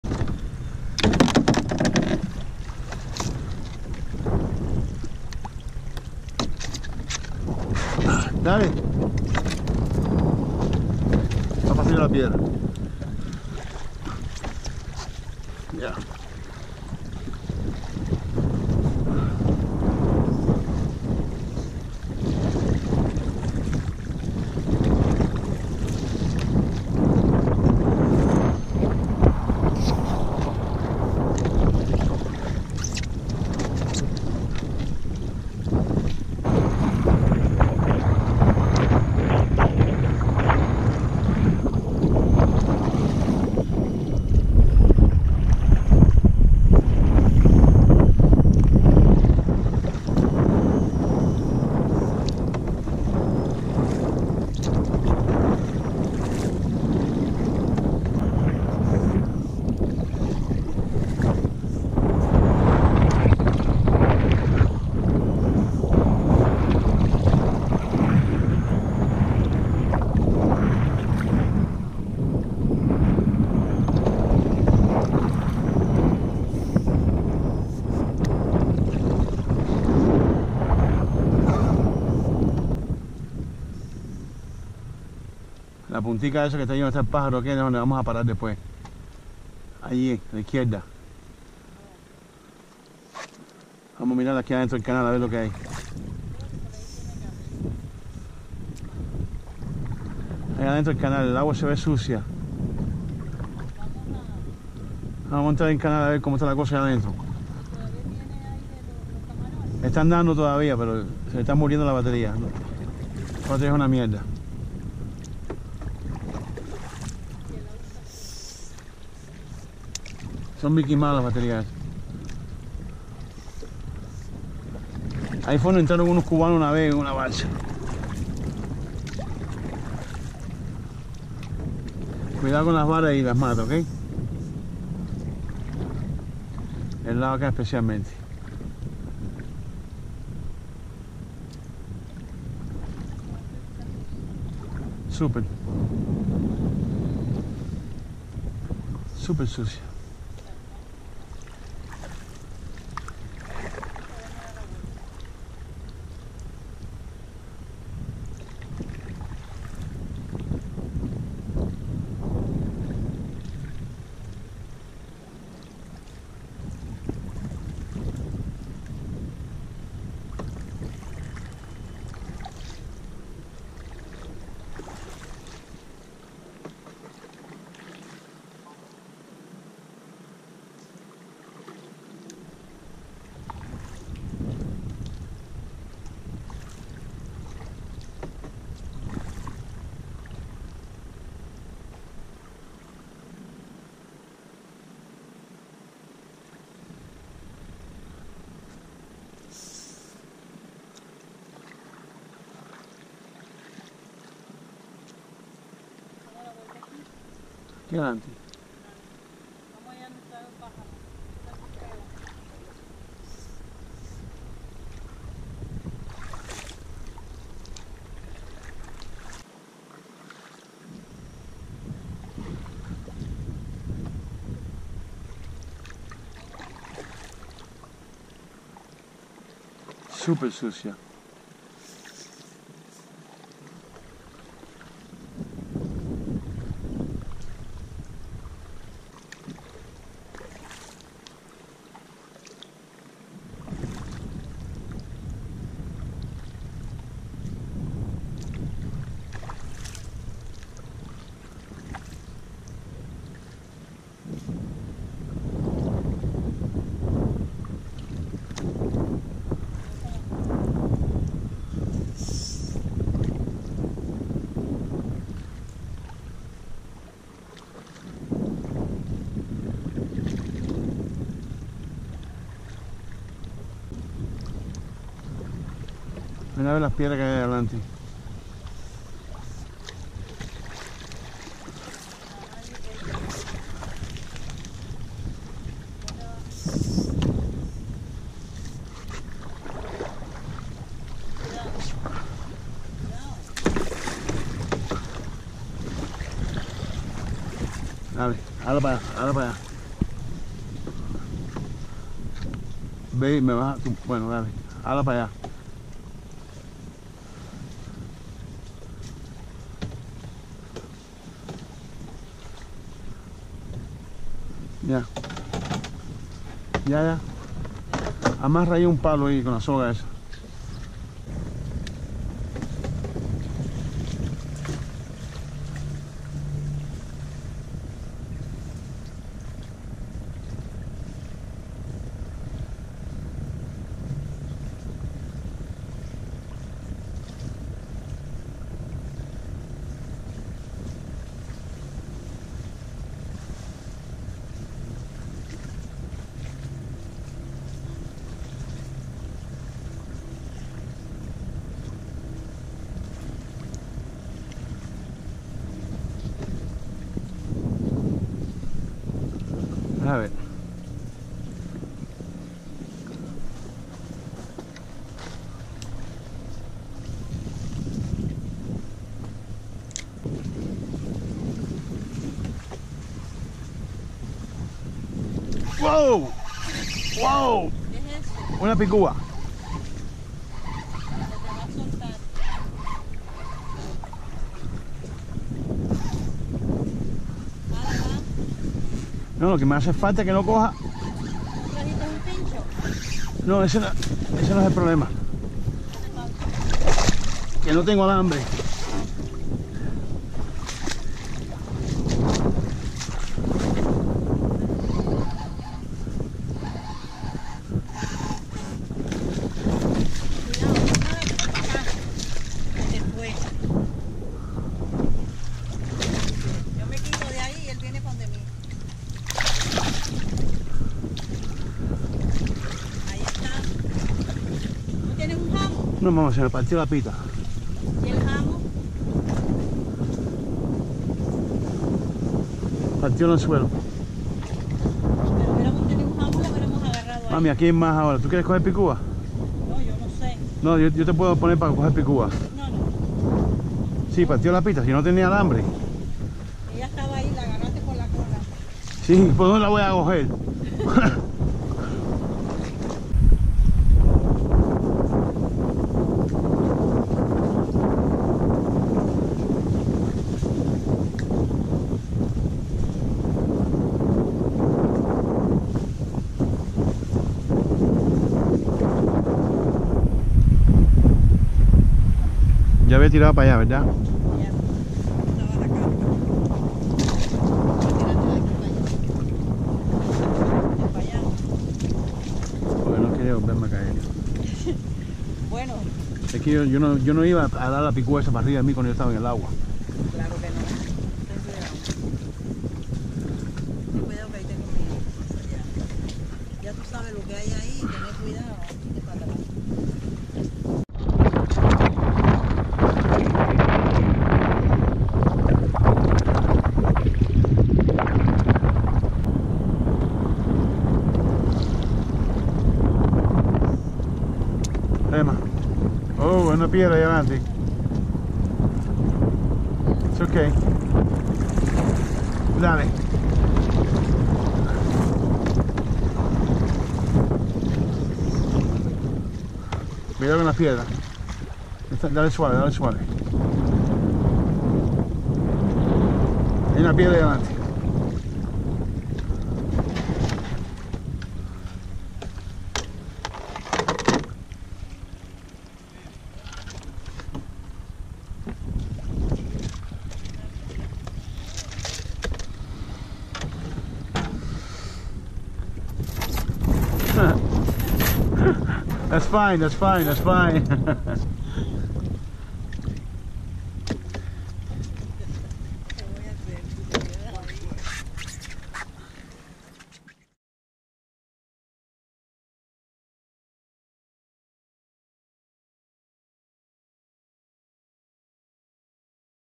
Deering Point Canoe Trip Feb 2019
First Canoe ride for my girl out of Deering Point. Very windy day, not much  fish at all and lost 2 Cuda's at the water's edge, Only one on film though. So many mistakes on that